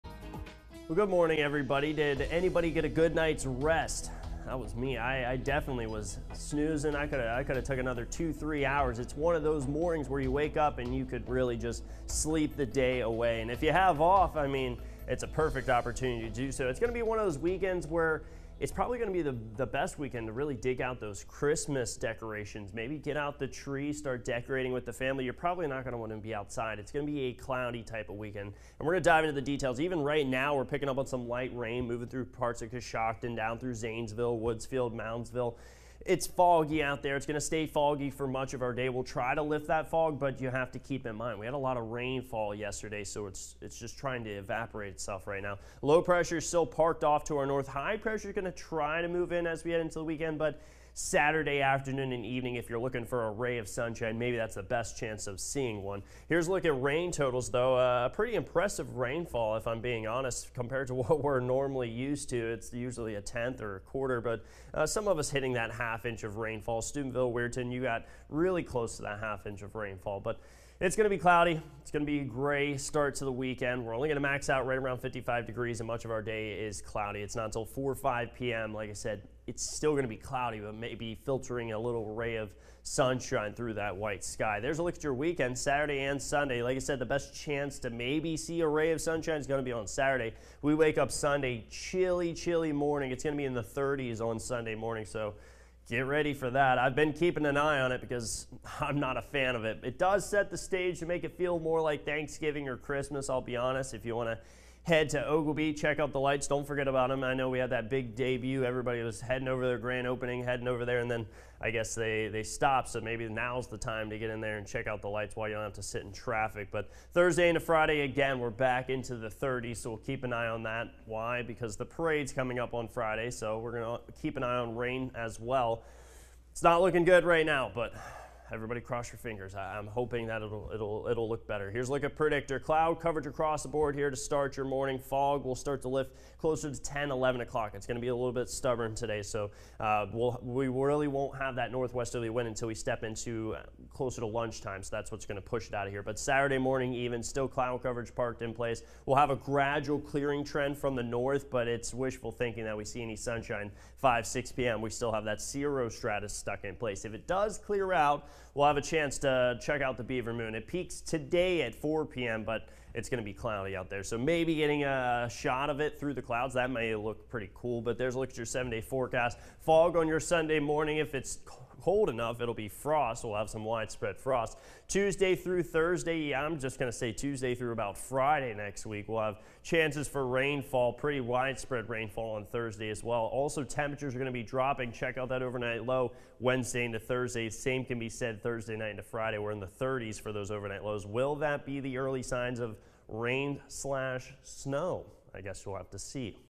Cloudy start to the weekend
Meteorologist Tyler Vangi breaks down the weekend in the middle of November 2024